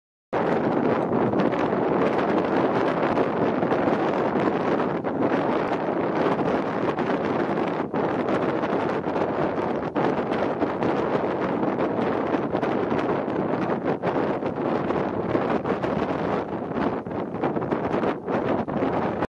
File 10 11 16, 8 20 49 pm
APC Mad Max winning the 2015 APC King of the Gulf passage race.